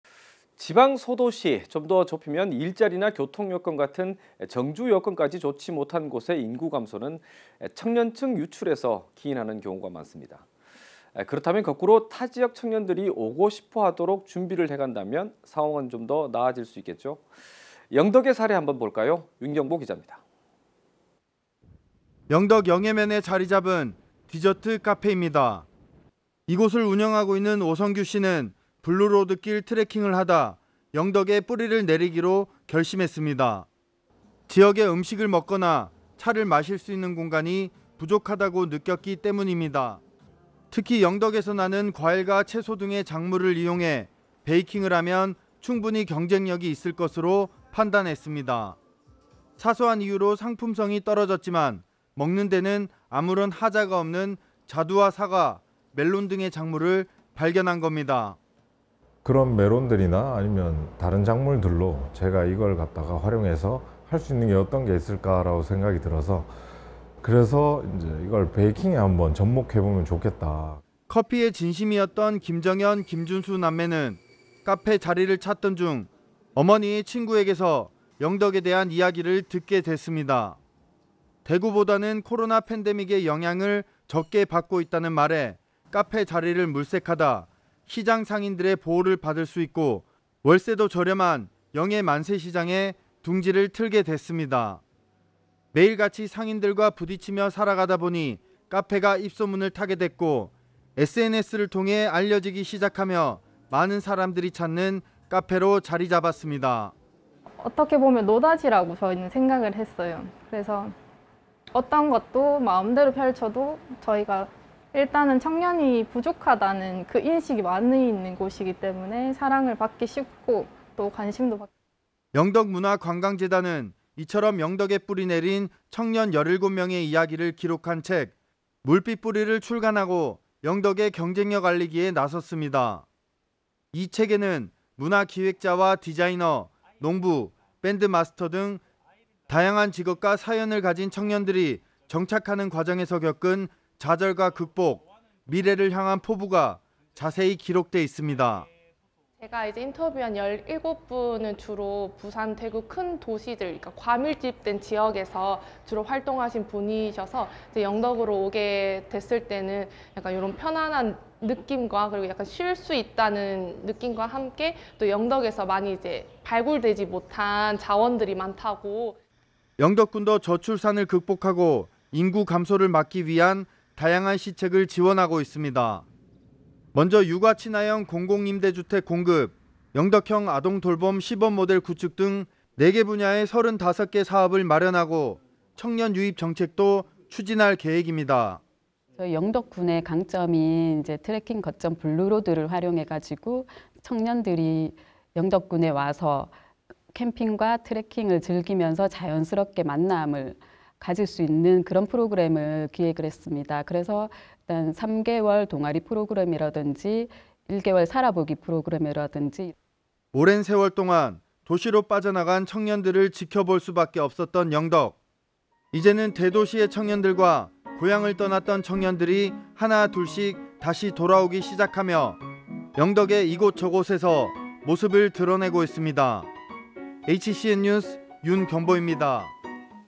영덕에 뿌리 내린 청년들..."인구 감소 막는다" / HCN경북방송
지방 소도시, 좀 더 좁히면 일자리나 교통 같은 정주 여건까지 좋지 못한 곳의 인구 감소는 청년층 유출에서 기인하는 경우가 많습니다.

그렇다면, 거꾸로 타지역 청년들이 오고 싶어하도록 준비를 해간다면 상황은 좀 나아질 수도 있겠죠?

영덕의 사례 한번 볼까요?

윤경보 기잡니다.